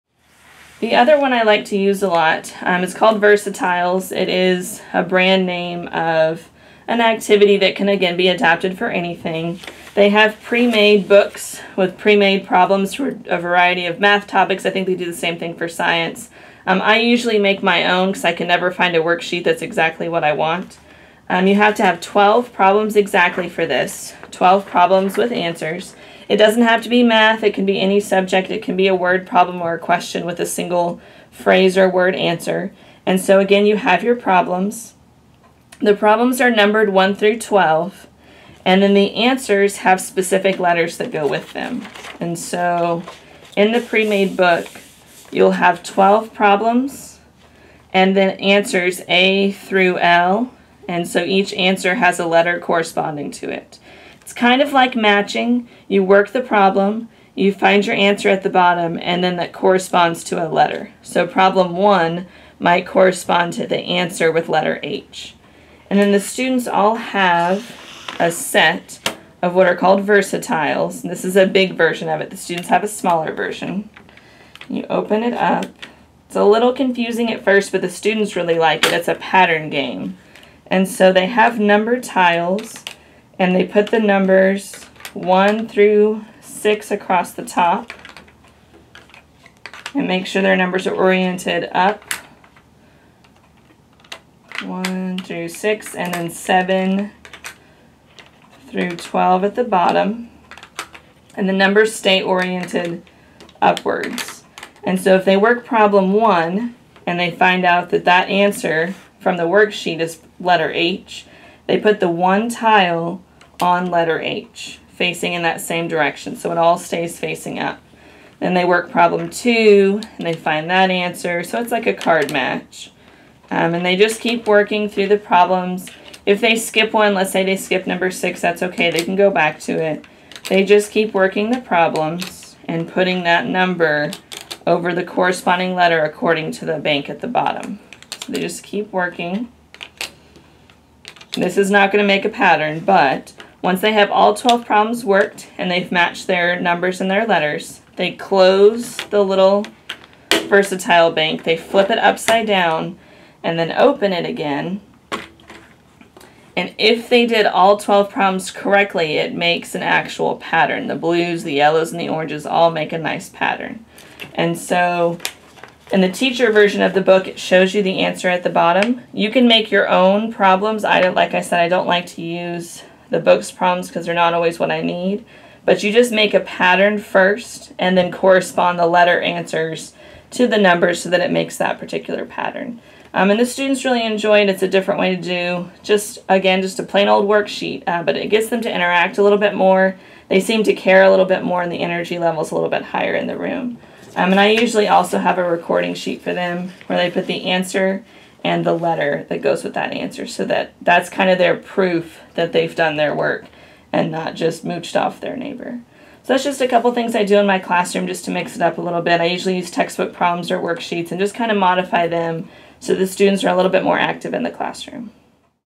Active U - Practice Versa Tiles for Pre Algebra
Professor of Math, Christina Leone utilized Versa Tiles.